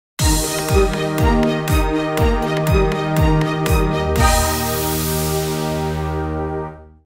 Sigla Meeting 2018
Così il videomaker Massimo Toniato ha “messo in movimento” il manifesto del meeting18 con una brevissima sigla che accompagnerà i nostri video di qui fino al Meeting. Enjoy it!